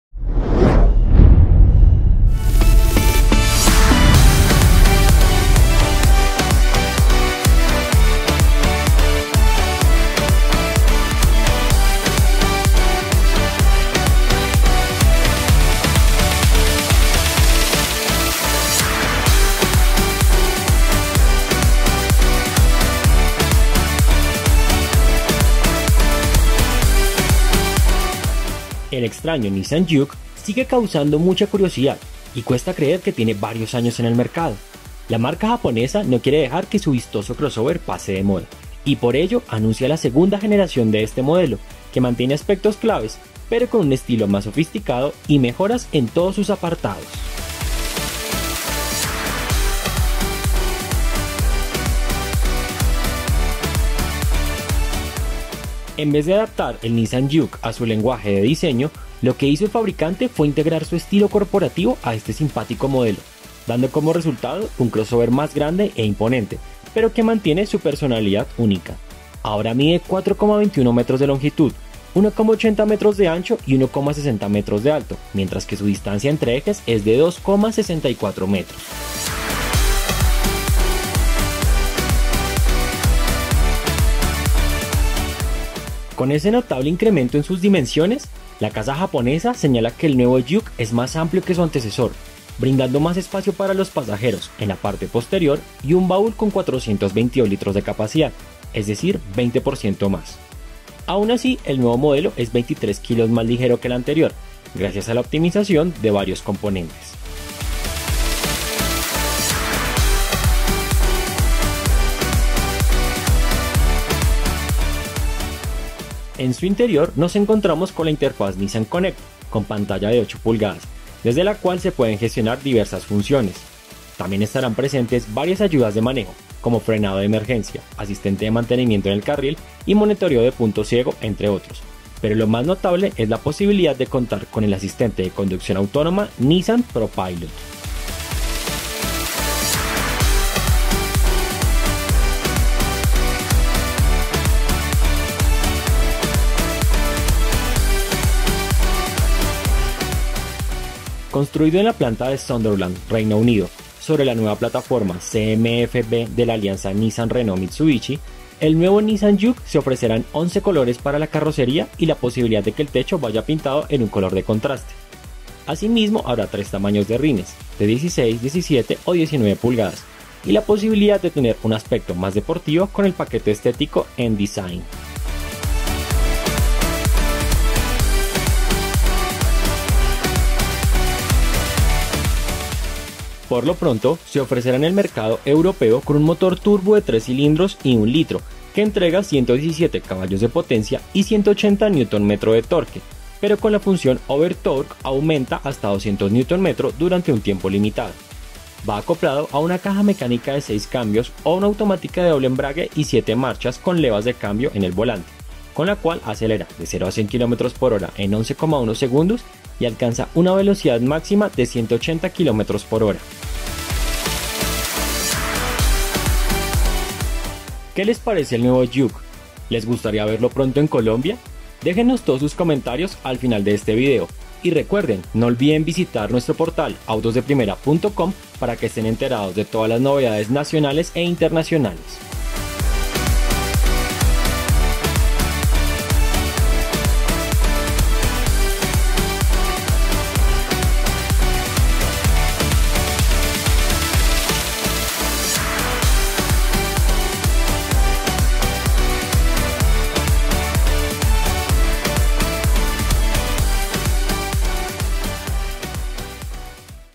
🔥 Nissan Juke 🔥¿Sigue siendo un CROSSOVER exótico?
Nuevo Nissan Juke (2020), el controvertido crossover de la marca japonesa se renueva. Encuentra en este los detalles más destacados. ¿Les gustó la nueva generación? Leemos sus comentarios aquí ⬇⬇⬇